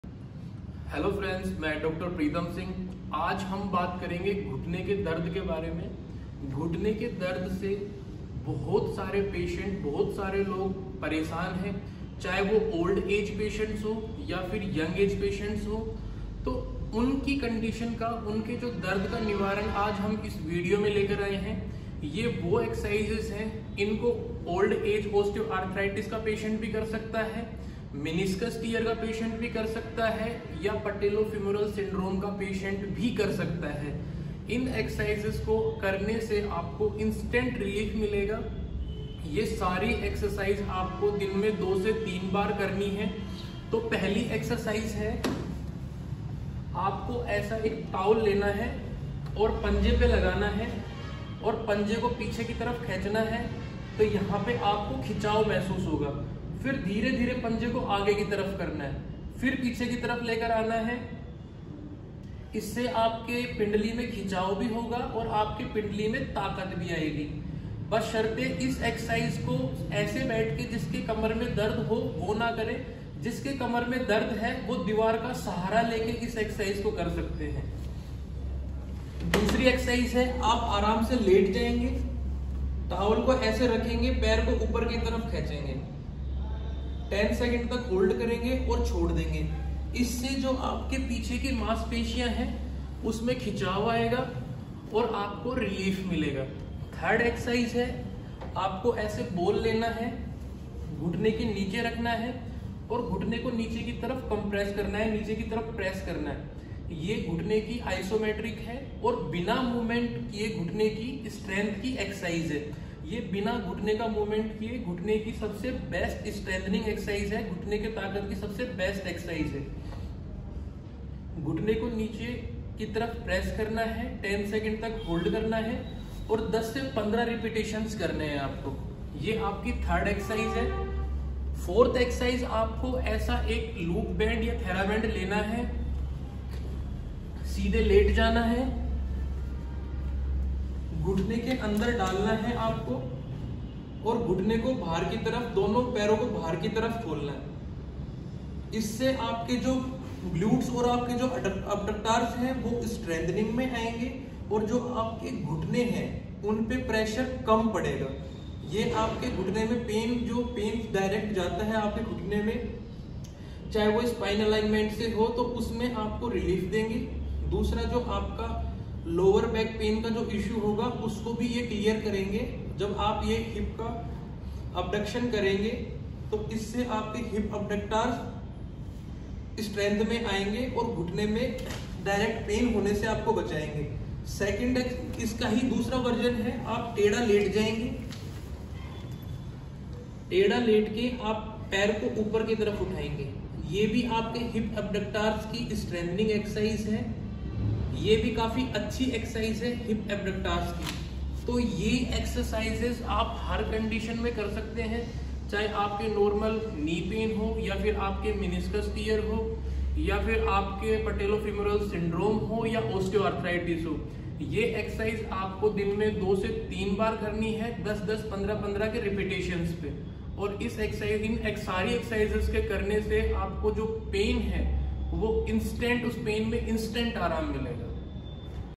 Best exercises for Knee pain, Osteoarthritis, chondromalacia patellae | DR. PRITAM SINGH
In this video i  Dr . Pritam singh MPT (musculoskeletal and sports Disorder )will explain you about knee pain and their best exercises for relieve in pain